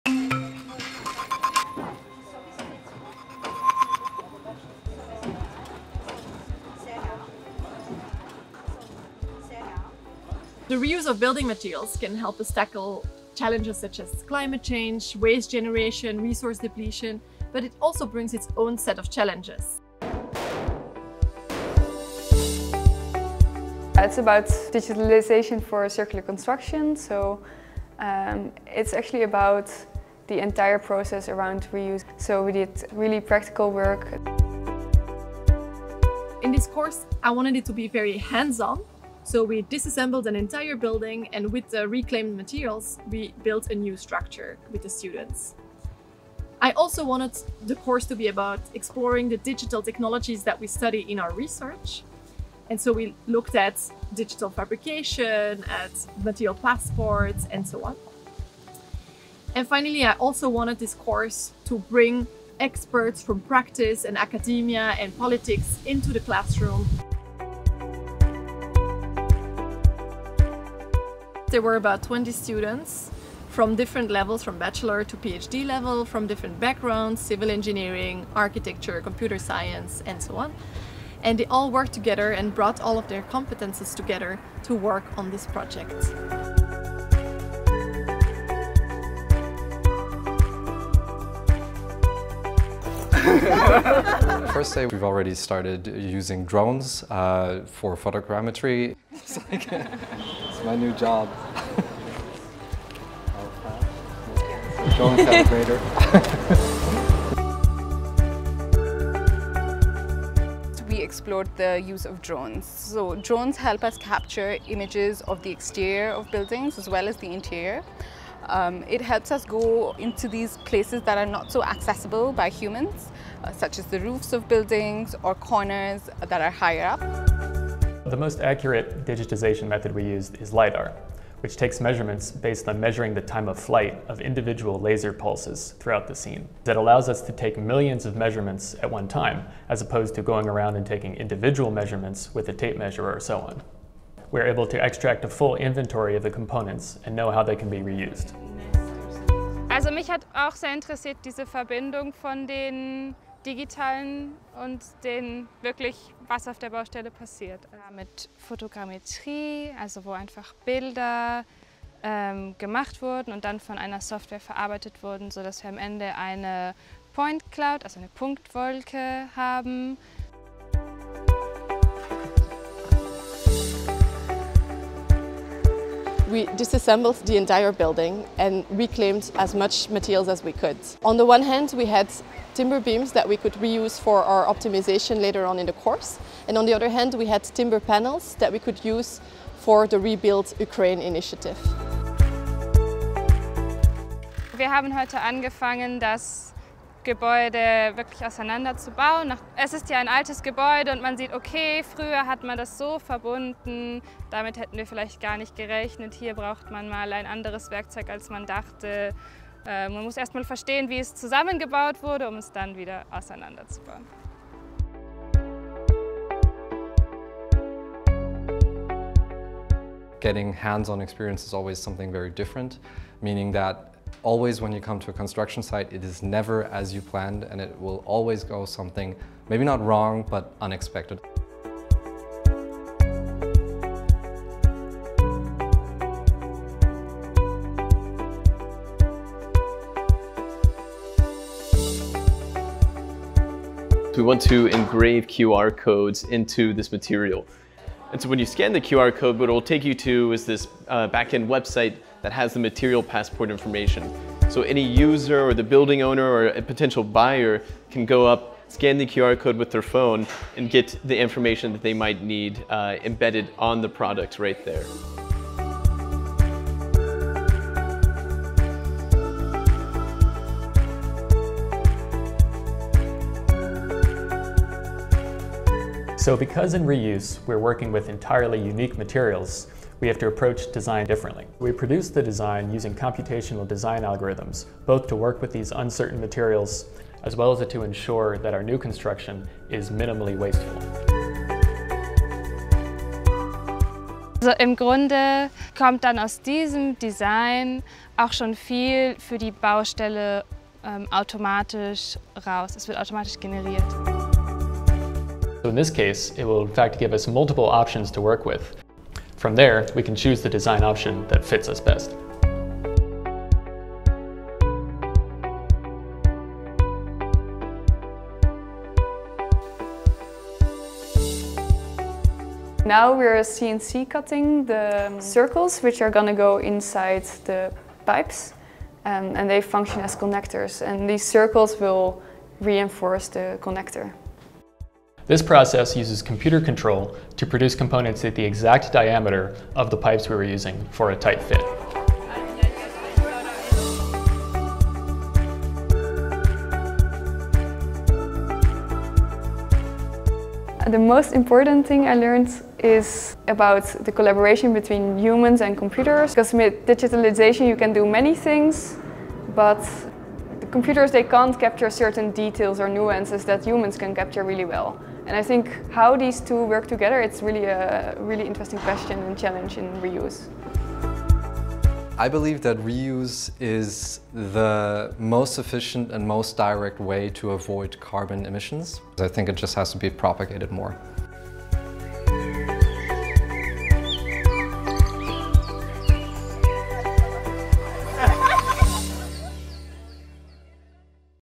ETH Zurich Summer Course 2023 - Digitalisation for Circular Construction - Prof. De Wolf
How can we bring together two worlds that are often too distinct: low-impact construction and digital innovation? Exploring digital technologies, students learn about circular construction (e.g., reuse) hands-on through the disassembly of an entire building to the fabrication of a new one solely with the reused materials.

The film was selected to be screened at the Global Science Film Festival by the Swiss Science Film Academy.

Directors: Catherine De Wolf, Elio Stamm, Anna Buser

Interviewees: Catherine De Wolf, Josien de Koning, Deepika Raghu, Matthew Gordon, Brandon Byers, Leonhard Schönfelder, Miriam Sonnak

Images: Elio Stamm, Catherine De Wolf, Anna Buser, Ian Oggenfuss, Sara Hofstetter, Artan Hajrullahu, Students & Assistants

Special Thanks: Rotor, Wiederverwerckle, Hinderer Liegenschaften, Studio Tom Emerson, Raplab, Festool, Salza, Innovedum Grant, CEA Instructors, Teaching Assistants & Students